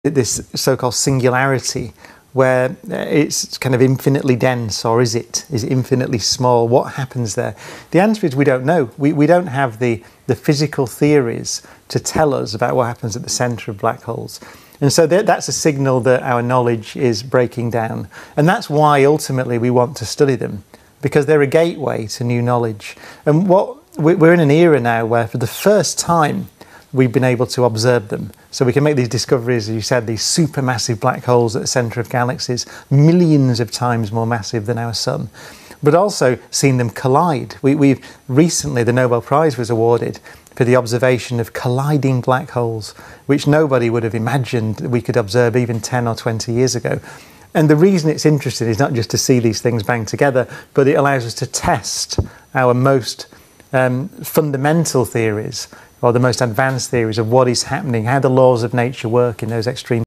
Singularity Black Hole Explained Brian Cox
Singularity black hole explained Physicist Brian Cox. What is inside the black hole, The singularity at the center of a black hole is the ultimate no man's land: a place where matter is compressed down to an infinitely tiny point, and all conceptions of time and space completely break down. Related topics